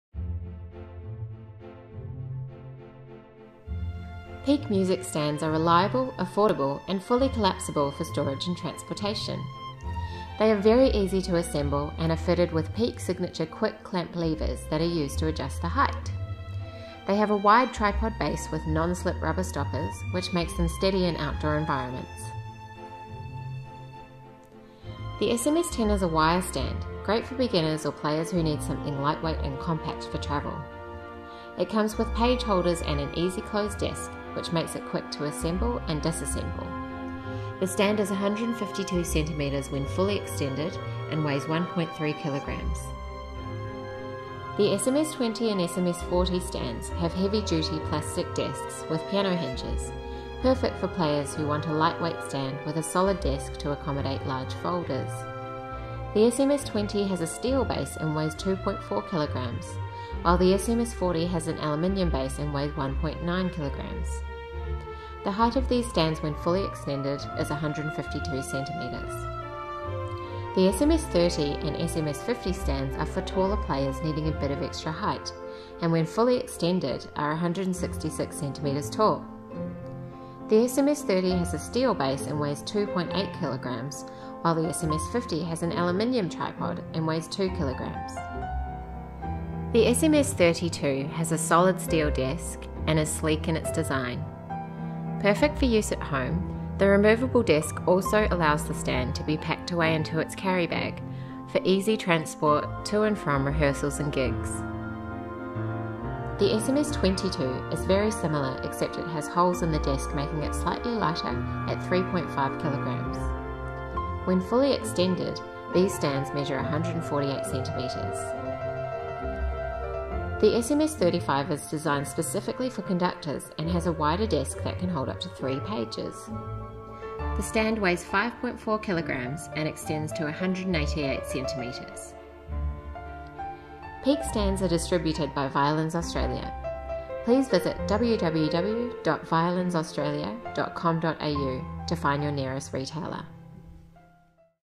Peak Music Stands Range
In this video we go through the range of Peak Music Stands. Currently we are supplying the following stands:

Peak SMS-10 Wire collapsible
Peak SMS-20 Plastic desk with piano hinges (Steel frame)
Peak SMS-40 Plastic desk with piano hinges (Aluminium frame)
Peak SMS-30 Plastic desk, extra tall height (Steel frame)
Peak SMS-50 Plastic desk, extra tall height (Aluminium frame)
Peak SMS-32 Steel removable desk
Peak SMS-22 Steel removable desk with holes
Peaks SMS-35 Conductor stand with steel desk